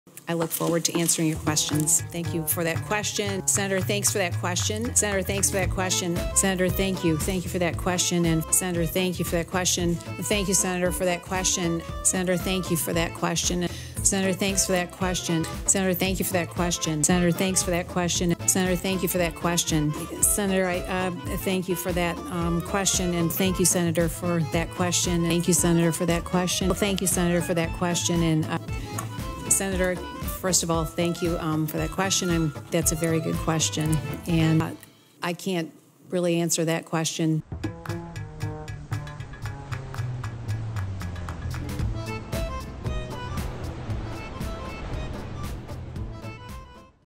Betsy DeVos Appreciates a Good Question
She wont necessarily answer your question, but she is thankful for them.

By
nolan culver
joey cruz

FOLLOW NOLAN!

FOLLOW JOEY!